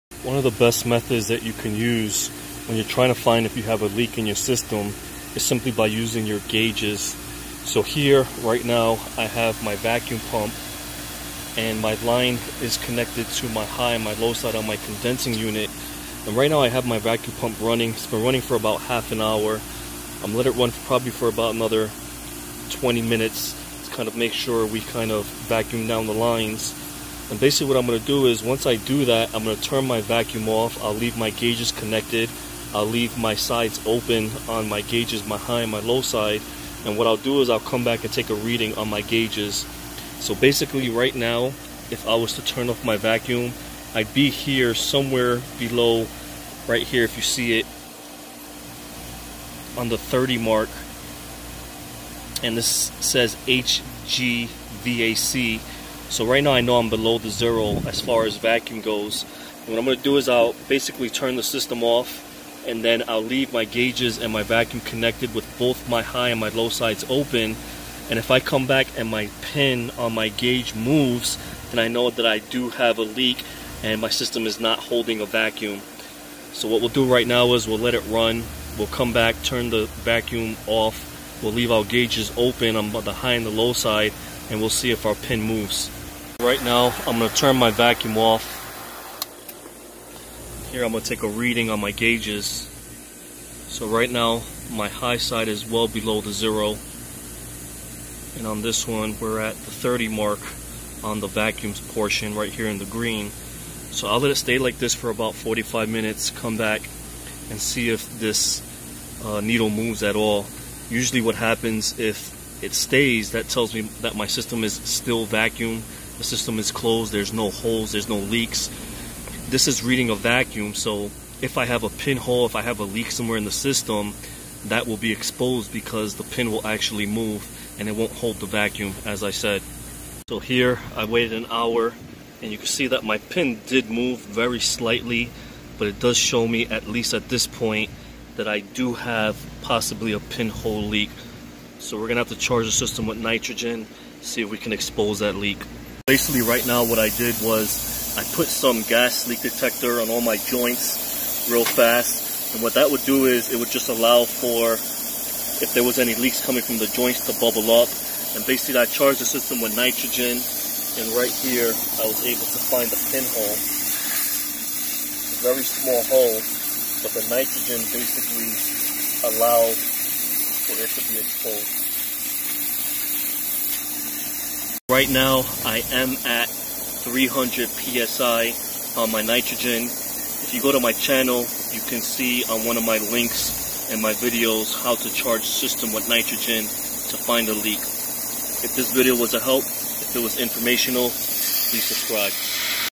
How to FIND Refrigerant Leak. Easy Method!
The best way to find a leak in your system is to pull a vacuum. The guages will be used as a metering device. Once you determine their is a leak simply charge your system with nitrogen.